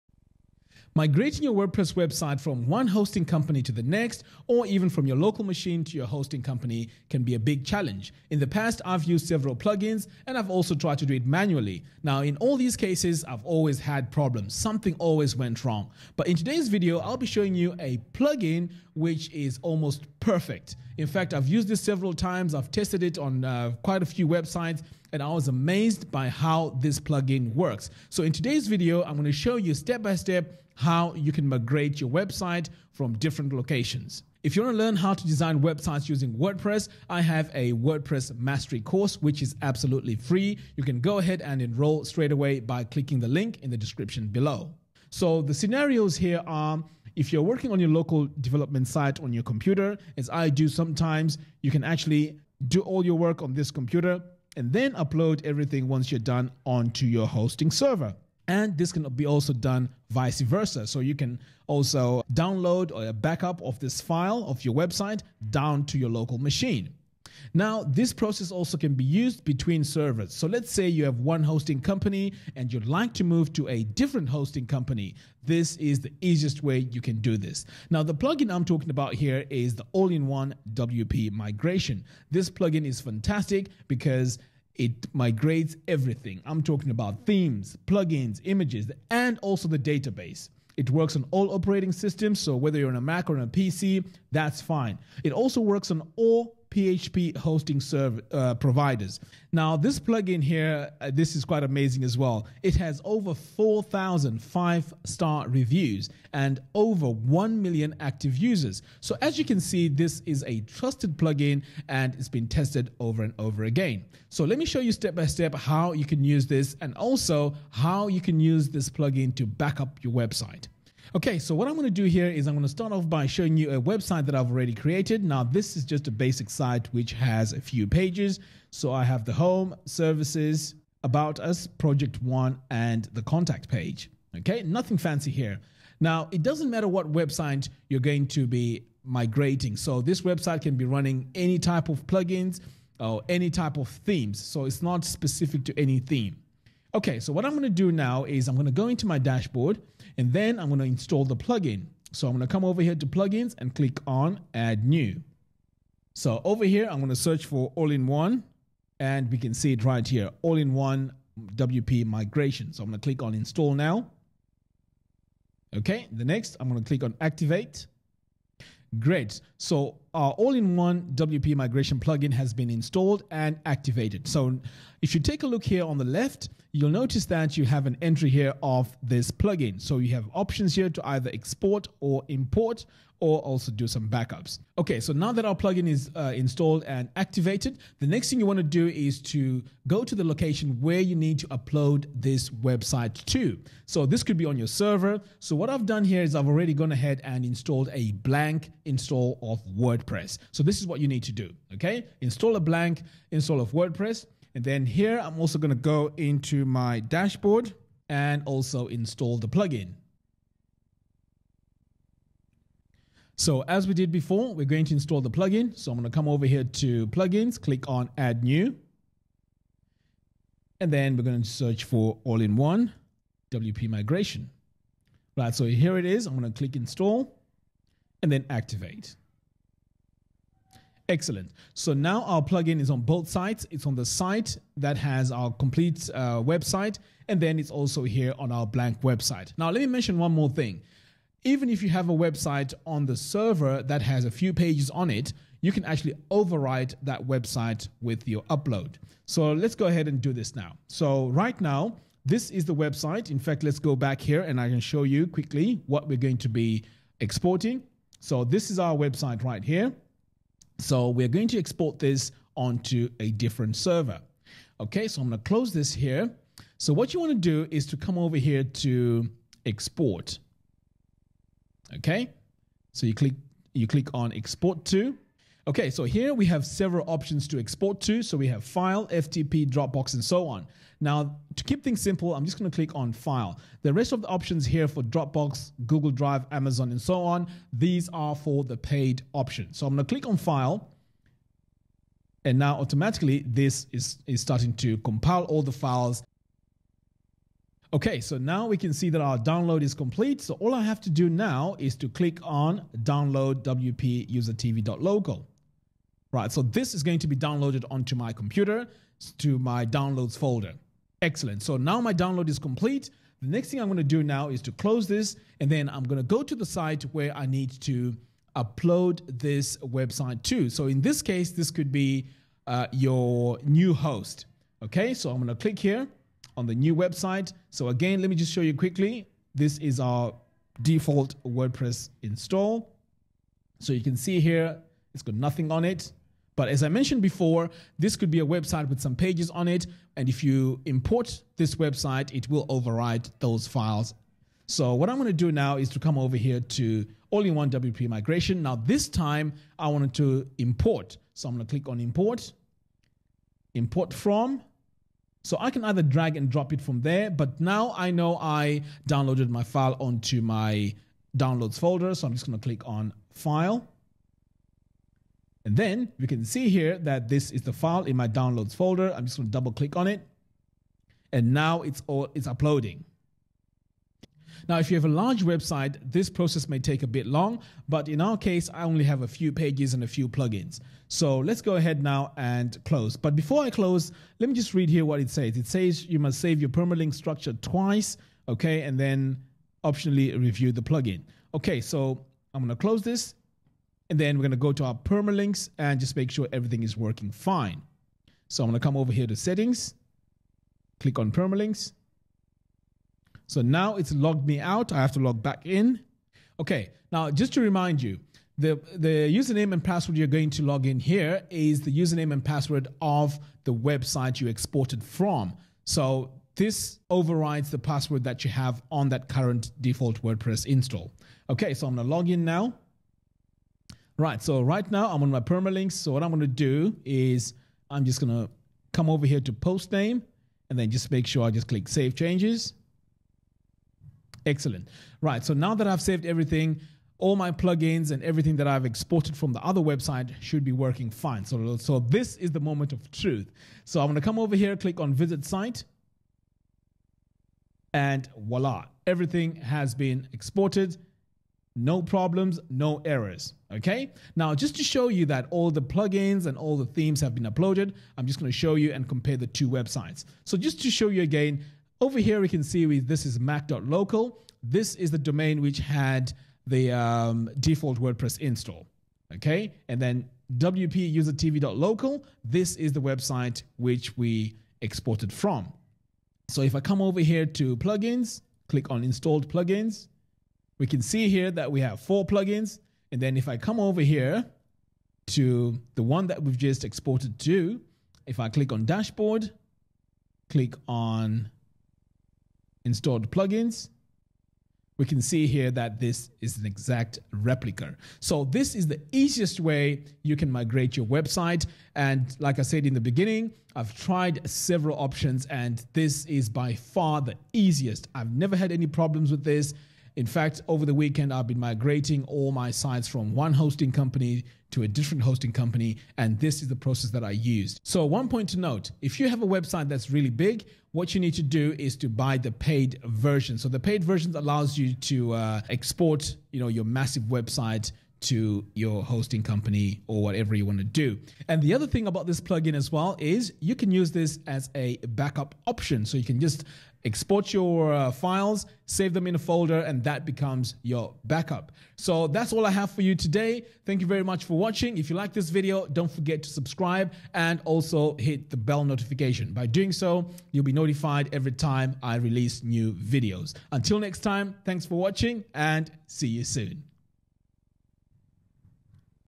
Moving WordPress site to new domain - All in one WP Migration
Moving WordPress site to new domain with all in one WP Migration.
In this video, I show you how to copy the WordPress site to a new host.